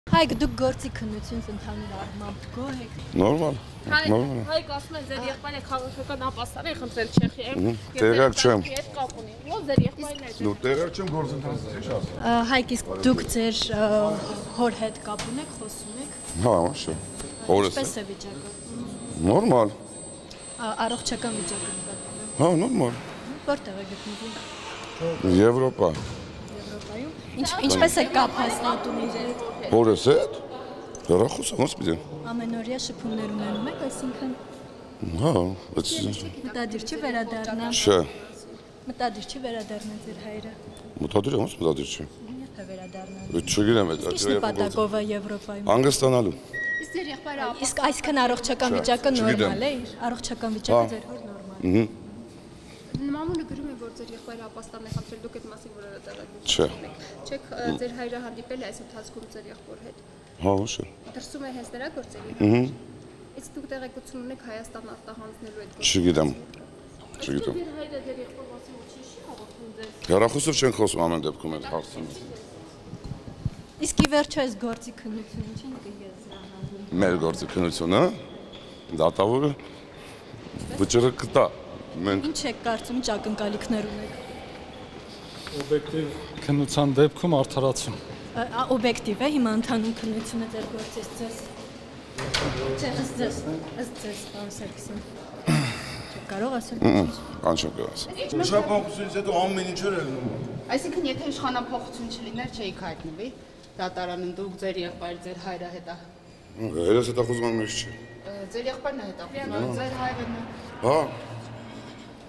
Ալեքսանդր Սարգսյանը հանգստանալու նպատակով գտնվում է Եվրոպայում. Հայկ Սարգսյան
Ալեքսանդր Սարգսյանը հանգստանալու նպատակով գտնվում է Եվրոպայում, հանդիպել է որդու՝ Նարեկ Սարգսյանի հետ